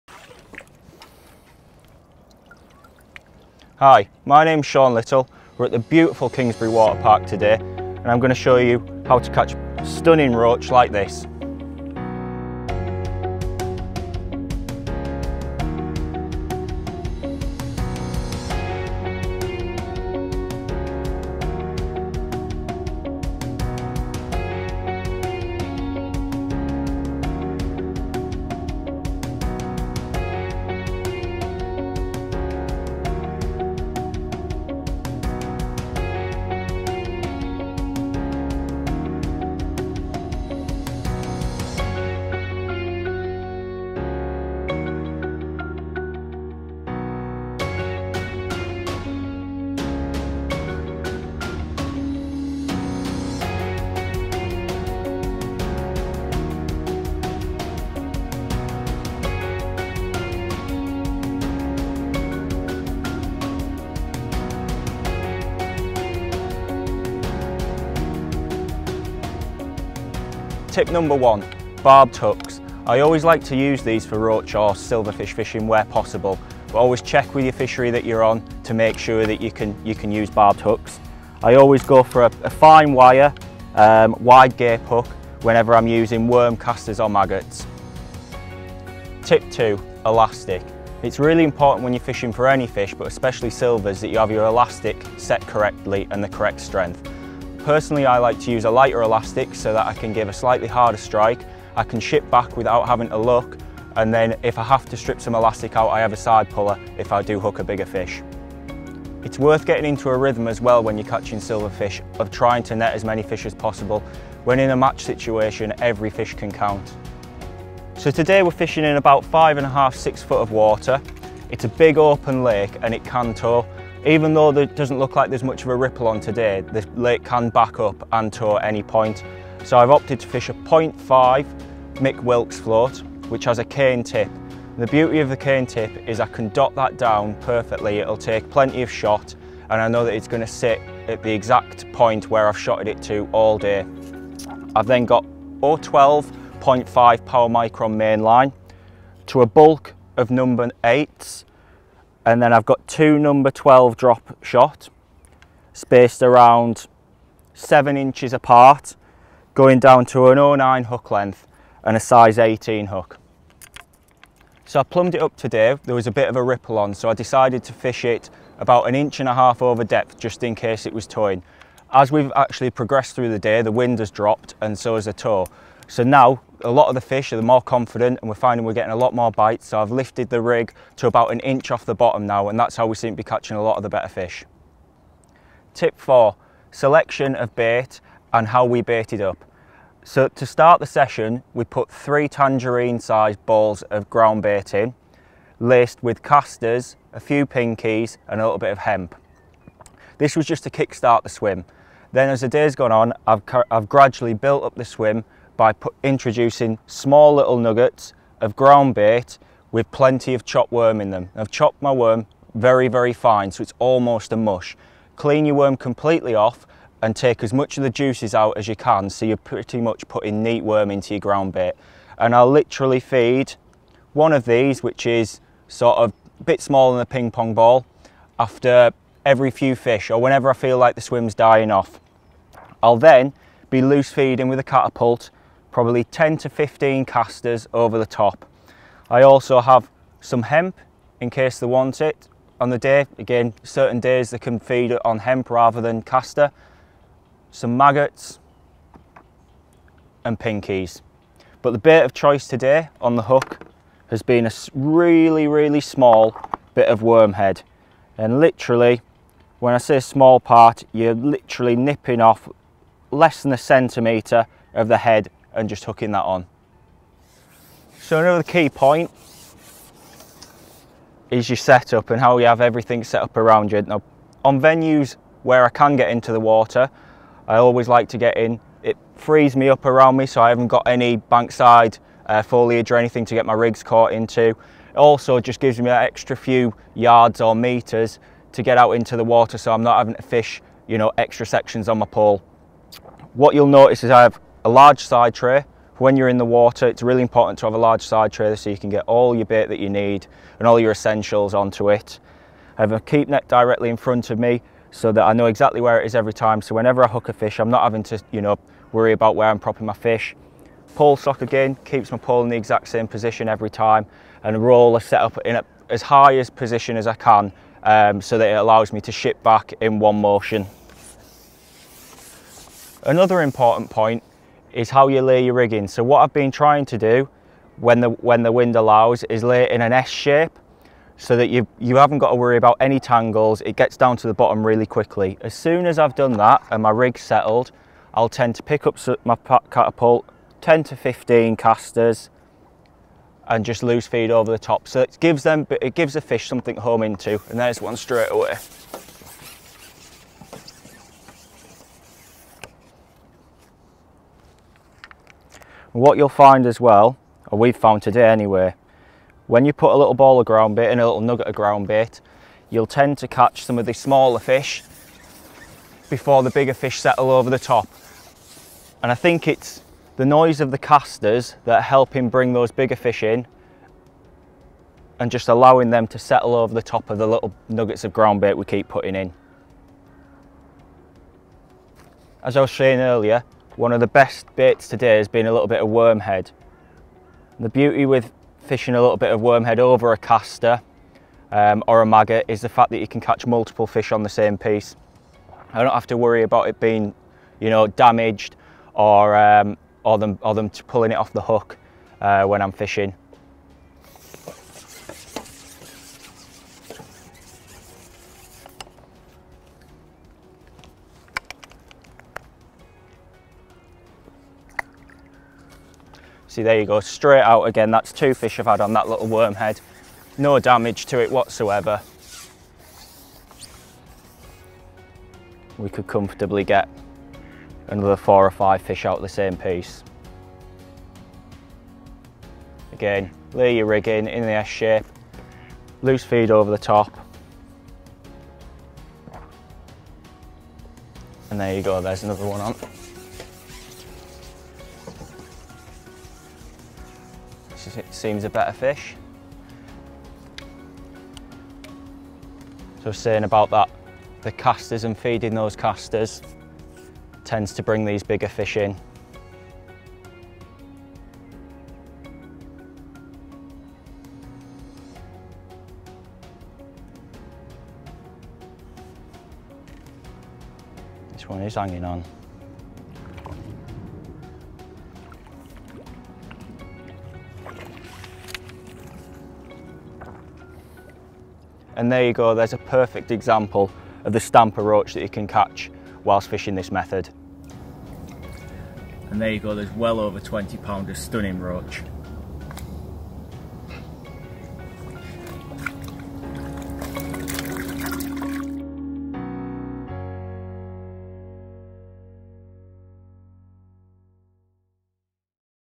Pole Fishing For Roach
Shaun Little from Matrix gives you a few tips to help with your roach fishing.

Shot Using my new Panasonic GH5

Edited using a iMac